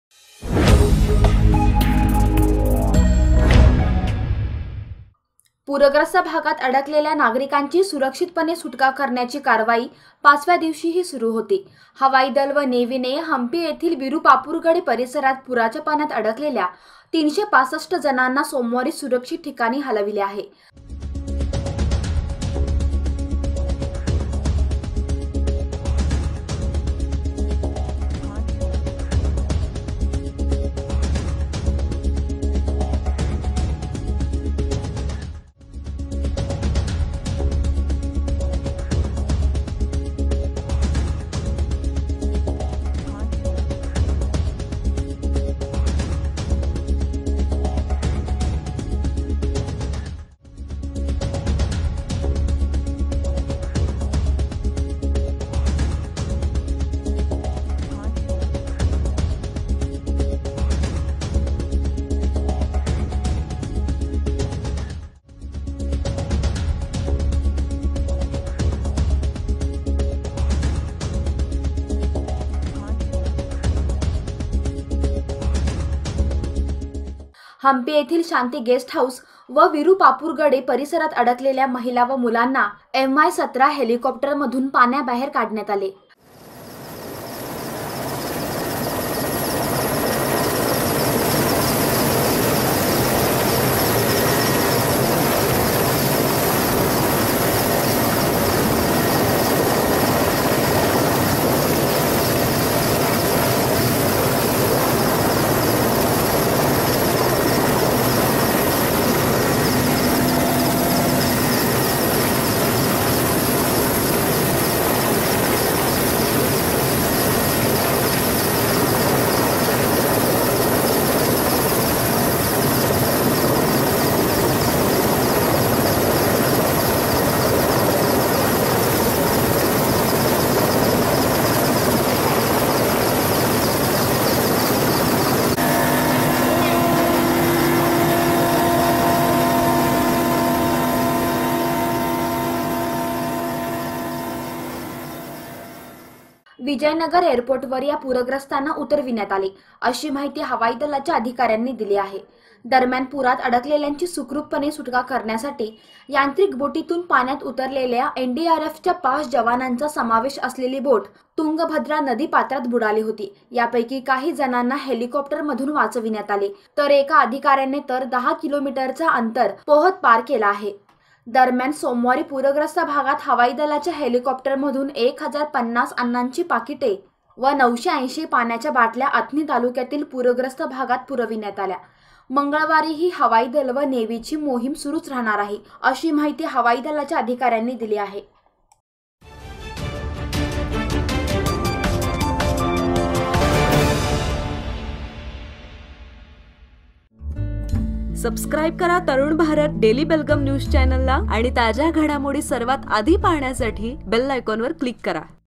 हंपी परिसरातून 365 जणांना सुरक्षितस्थळी हलविले | Hampi News | 13-8-2019
पूरग्रस्त भागात अडकलेल्या नागाfरकांची सुरक्षितपणे सुटका करण्याची कारवाई पाचव्या दिवशीहि सुरु होती. हवाई दल व नेव्हीने हंपी येथील विरुपापुरगड्डे पाfरसरात पूराच्या पाण्यात अडकलेल्या 365 जणांना सोमवारी सुरक्षित ठिकाणी हलविले आहे. हंपी येथील शांती गेस्टहाऊस व विरुपापुरगड्डे पाfरसरात अडकलेल्या माfहला व मुलांना एमआय-17 हेलिकॉप्टर मधून पाण्याबाहेर काढण्यात आले. विजयनगर एअरपोर्टवर या पूरग्रस्तांना उतरविण्यात आले, अशी माहिती हवाईदलाच्या अधिकार्‍यांनी दिली आहे.